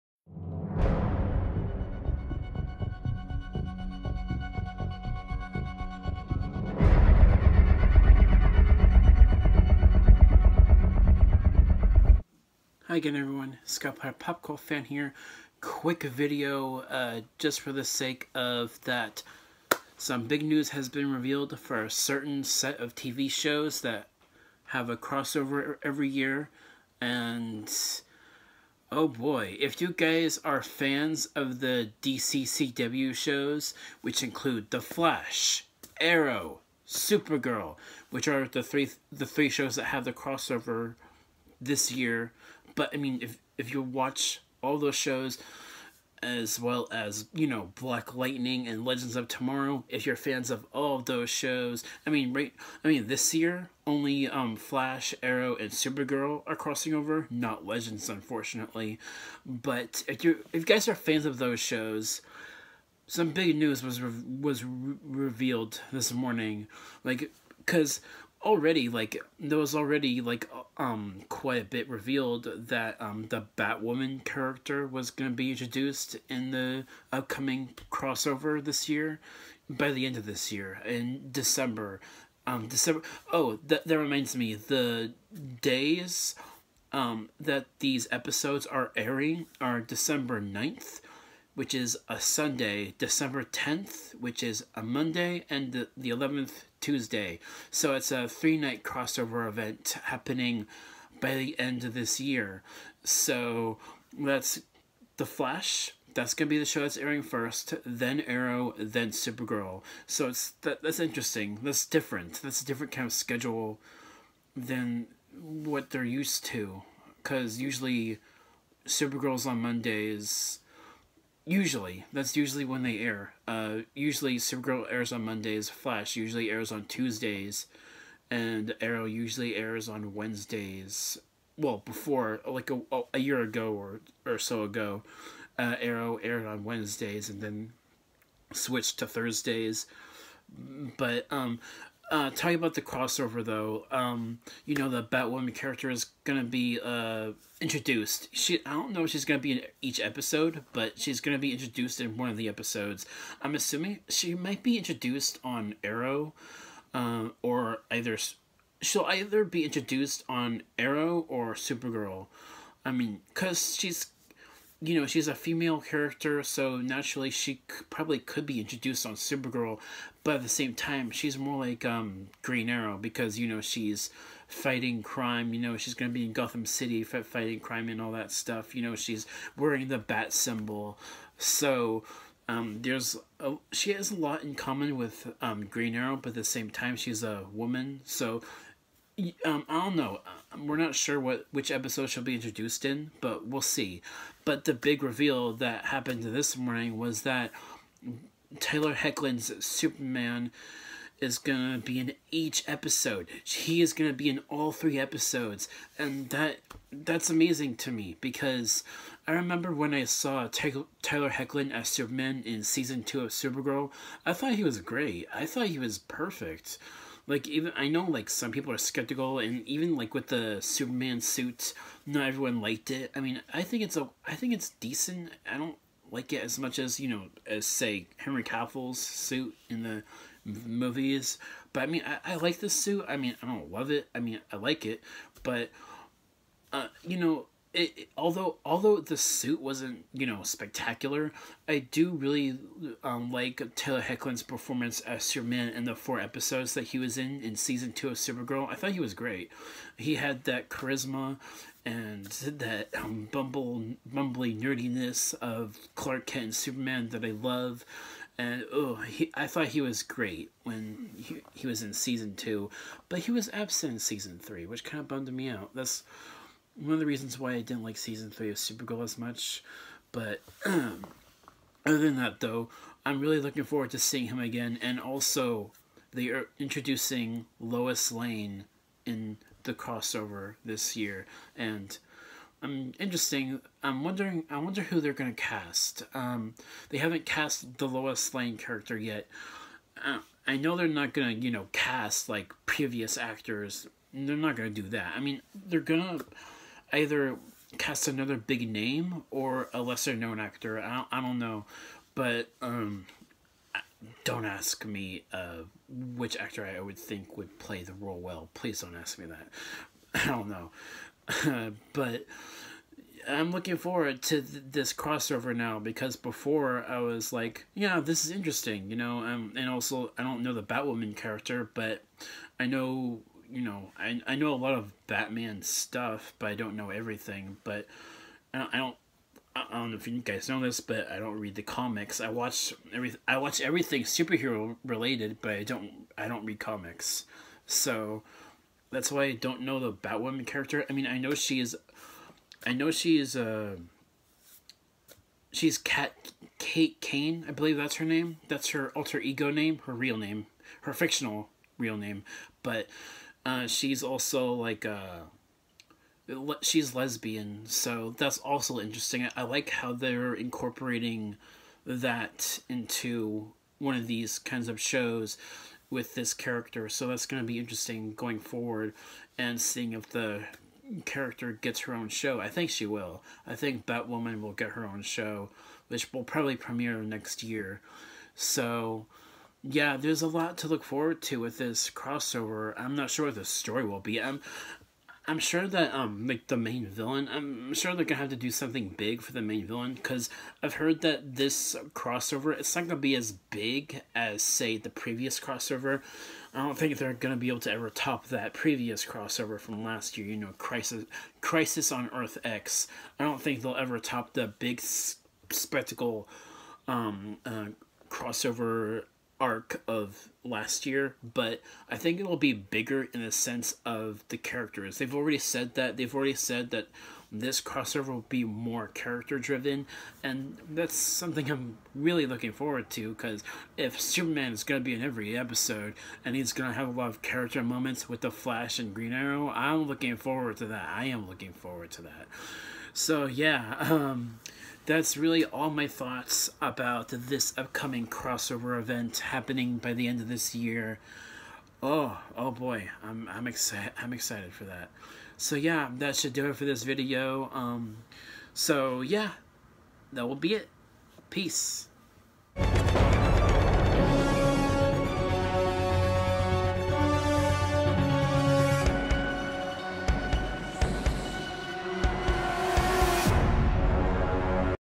Arrowverse Crossover 2018 Discussion
Hey guys, so after this will be my Pokemon X and Y review. I felt this announcement about the crossover was too important to not do a video about so here it is. Thank you all so much for watching and I’ll catch you all in the next video, peace!!